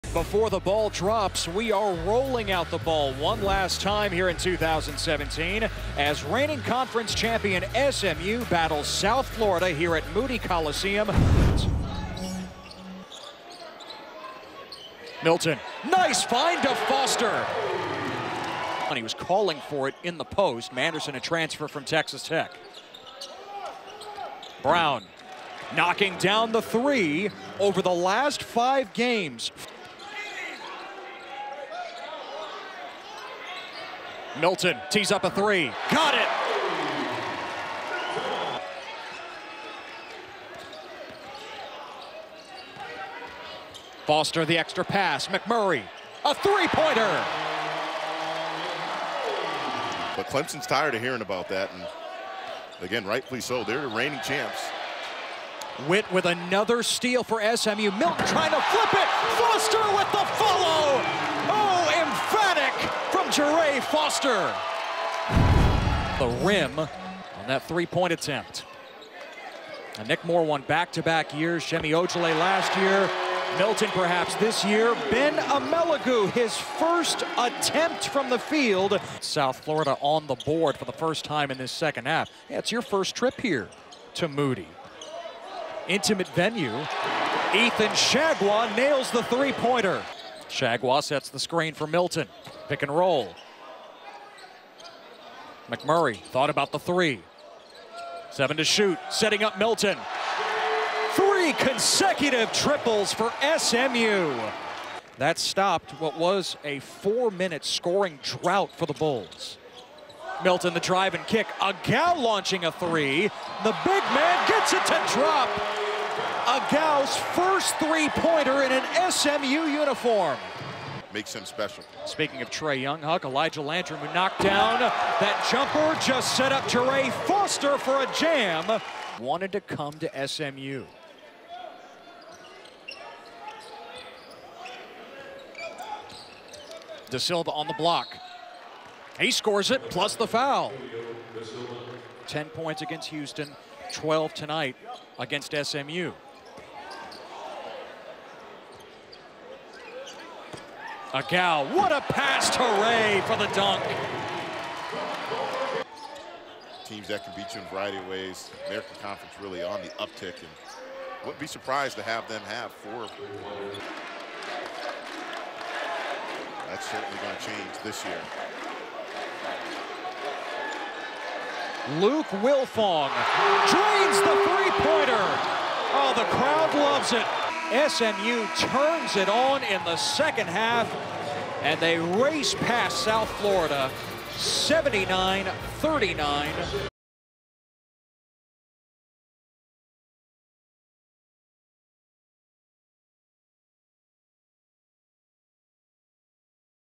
Men's Basketball Highlights - SMU 79 , USF 39
DALLAS (AP) -- Shake Milton had 14 points, two steal and two blocks and Jarrey Foster scored 12 points and grabbed eight rebounds to help SMU beat South Florida 79-39 on Sunday night for its 33rd consecutive home win.

Jahmal McMurray added 12 points and Jimmy Whitt had 11 points, on 5-of-6 shooting, and nine rebounds for SMU (12-3, 2-0 American Athletic Conference).

Despite the fact that USF (7-7, 0-2) missed 10 of its first 11 shots, including seven in a row to open the game, it was tied at 11-all with six minutes left in the first half. McMurray had five points and Foster scored four during a 13-2 run that made it 24-13 before the Mustangs went into the break with a 12-point lead. The Bulls made just 6 of 25 from the field -- tying their season low for field goals in a half -- and scored just 16 points before the break, their lowest scoring half of the season.

SMU made its first eight second-half shots, while USF missed its first 11, during a 26-1 run that gave the Mustangs a 54-17 lead with 12 minutes to go.

It was USF's lowest scoring output since an 83-40 loss at Connecticut on Feb. 12, 2014.